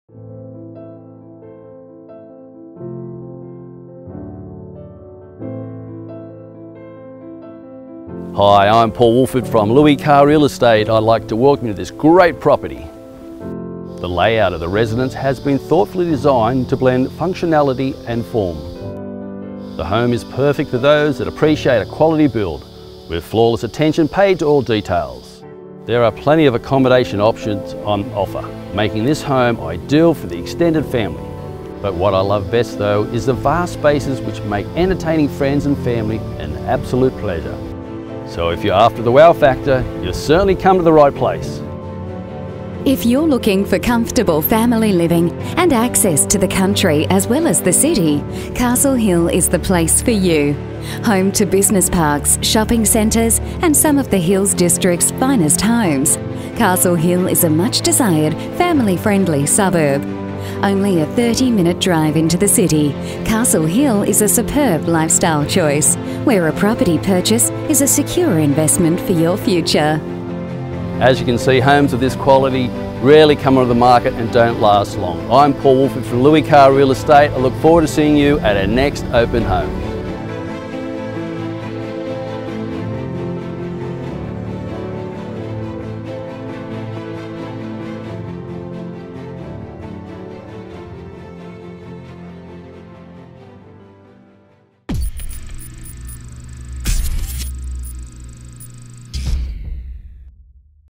18 Huntingdale Circle, Castle Hill
18 Huntingdale Circle, Castle Hill - listed for sale by Paul Woolford from Louis Carr Real Esate